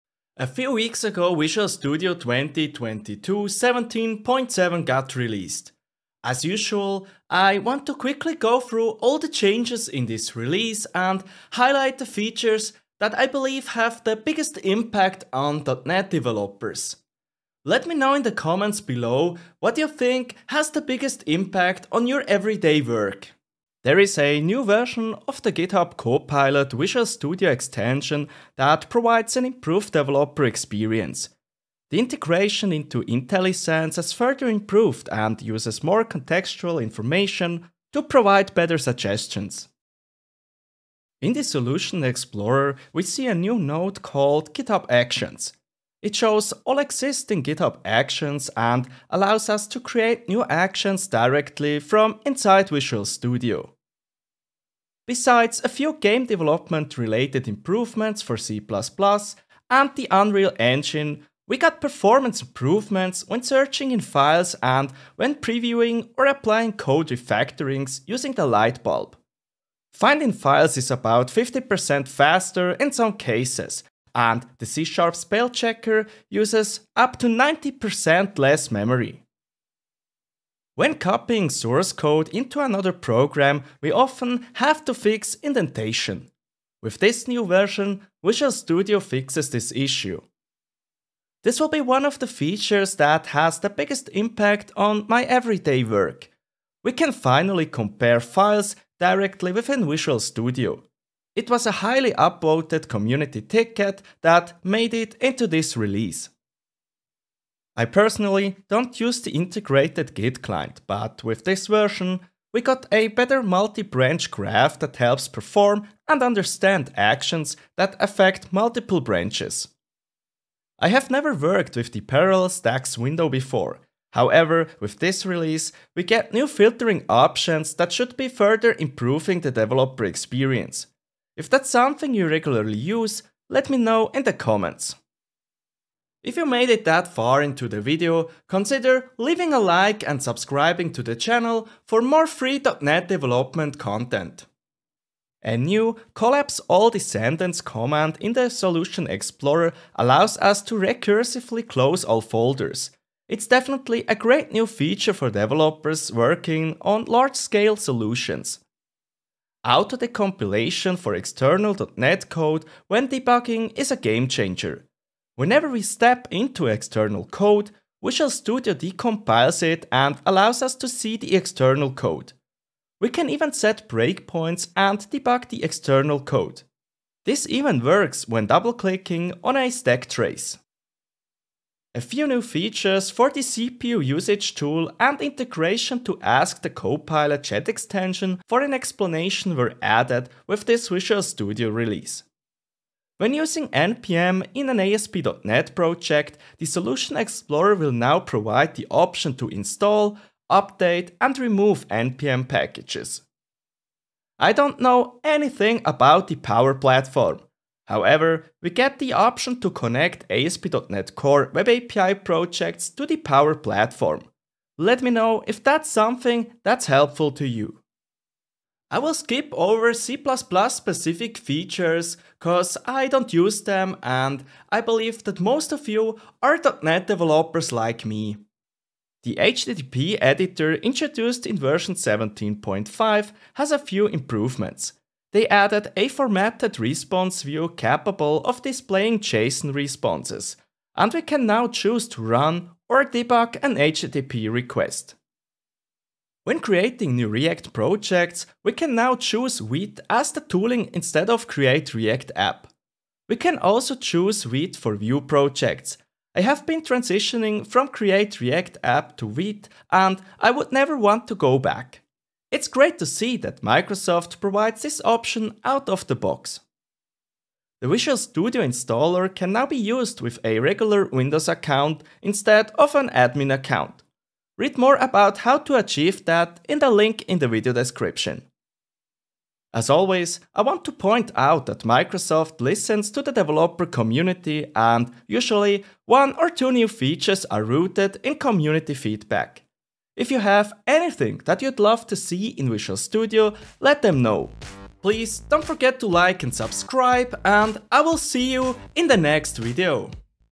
AUTODECOMPILATION IS HERE! | Visual Studio 2022 Update 17.7
A few weeks ago, Visual Studio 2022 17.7 was released. As usual, I want to quickly review the changes in this release and highlight the changes that I believe have the most significant impact on .NET developers.

📌 Configure Policy to run Visual Studio as non-admin (Enterprise)